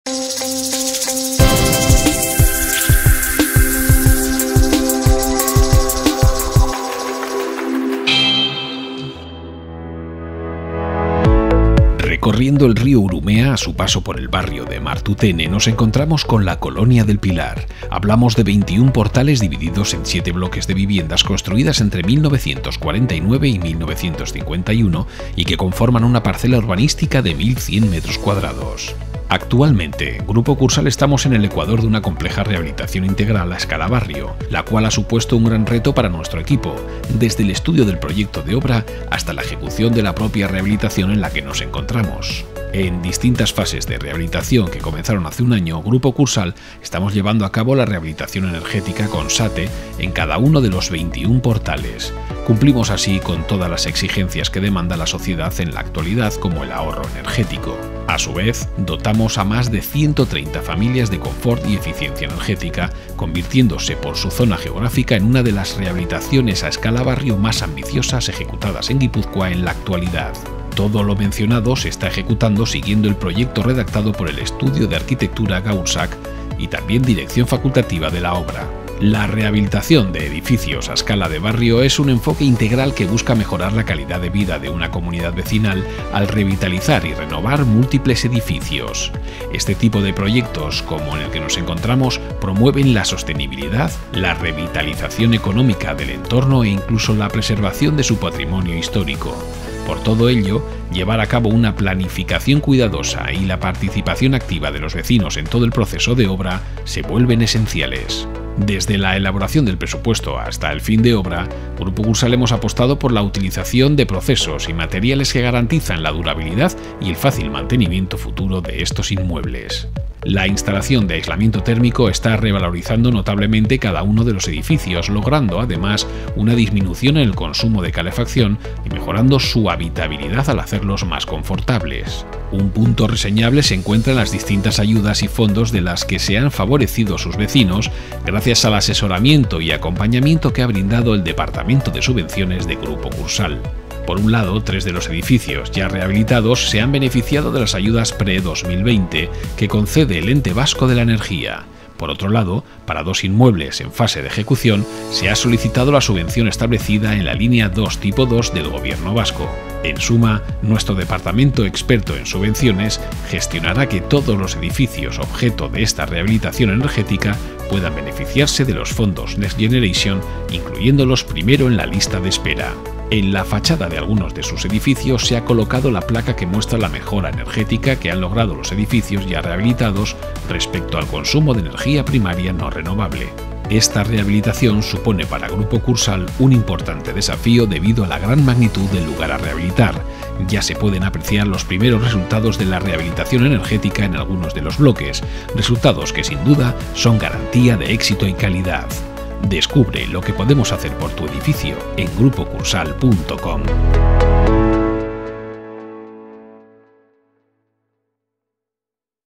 REPORTAJE. Futuro sostenible a escala barrio en la Colonia Pilar
GRUPO KURSAAL estamos en el ecuador de una compleja rehabilitación integral a escala barrio. Hablamos de 21 portales divididos en 7 bloques de viviendas construidas entre 1949 y  1951 y que conforman una parcela urbanística de 1.100m2.  Este tipo de proyectos promueven la sostenibilidad, la revitalización económica del entorno e incluso la preservación de su patrimonio histórico. Por todo ello, llevar a cabo una planificación cuidadosa, y la participación activa de los vecinos en todo el proceso de obra, se vuelven esenciales.